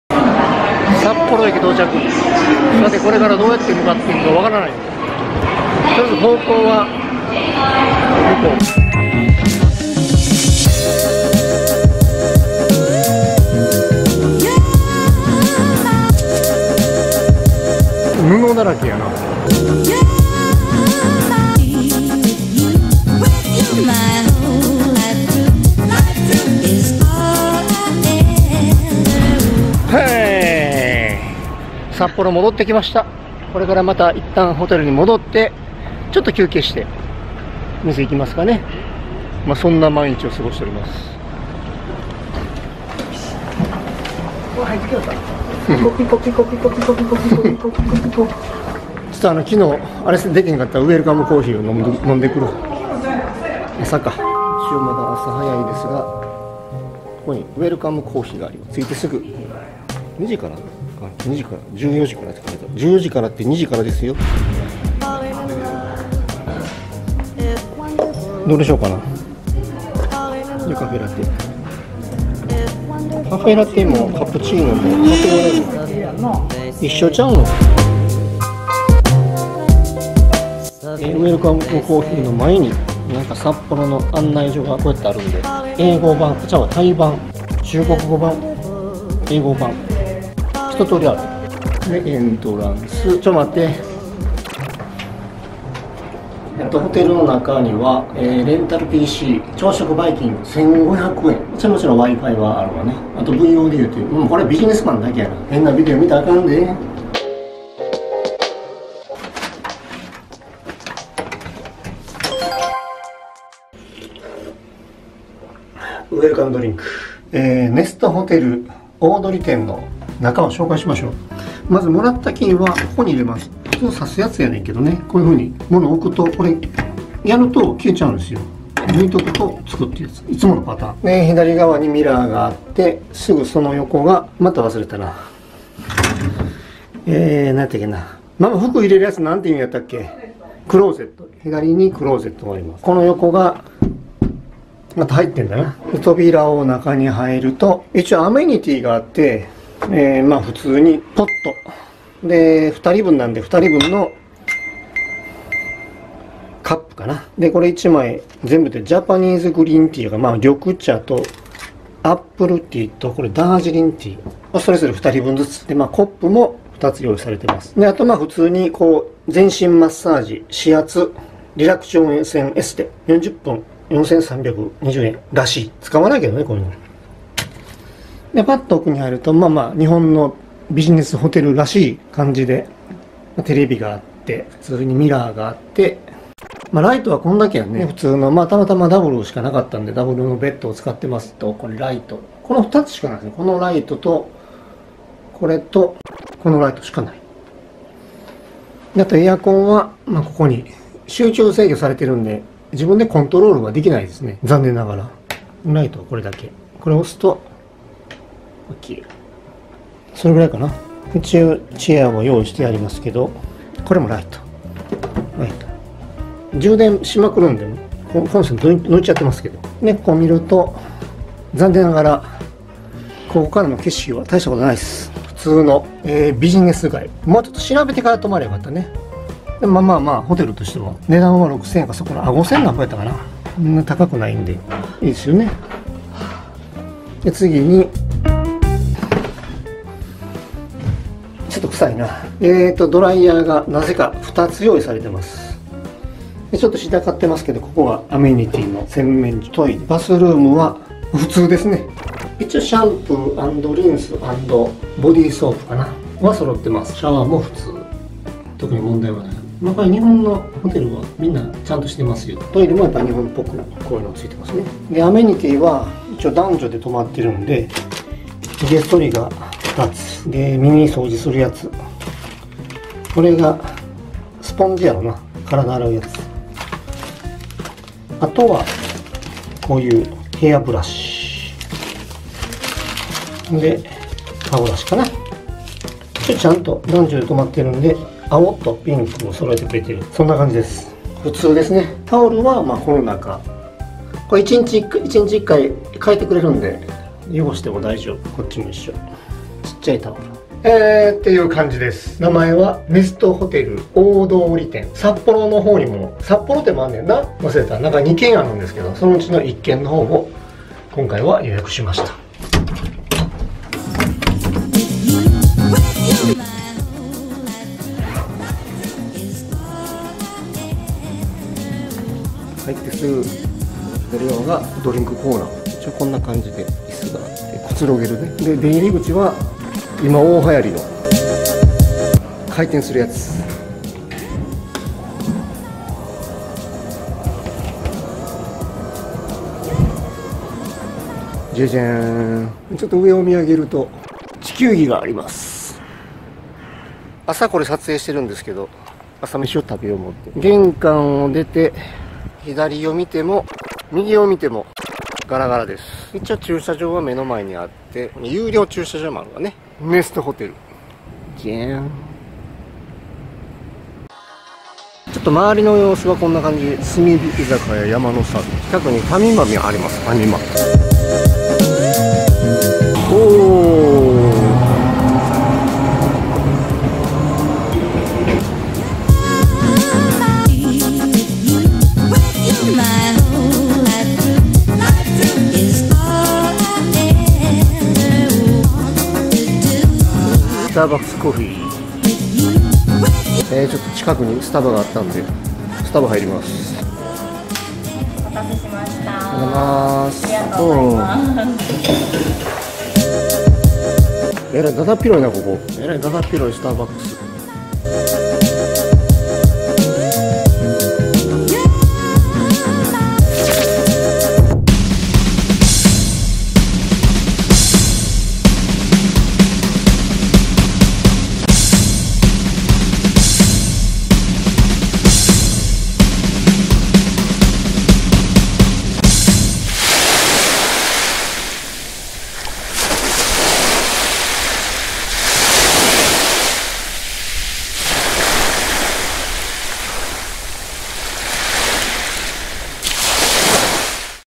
ネストホテル札幌大通に泊まってみた＠札幌で宿泊した宿だよ。
北海道札幌で宿泊した宿です。沖縄にも同系列の宿があるのを知ってる。そういったこともあり、普通に目に留まってしまって、ポチってしまった。名前の通り、札幌の大通駅近くにあり、ここをベースに札幌や小樽、苫小牧まで観光に出かけたのでした。

-Vlog 器材-
アクションカメラ１
アクションカメラ２
ダイブフィルター
撮影用カメラ
撮影用ビデオ（古い）
吸盤マウント
クリップ式雲台
自撮りハンドル
自撮り棒１
自撮り棒２
自撮り棒３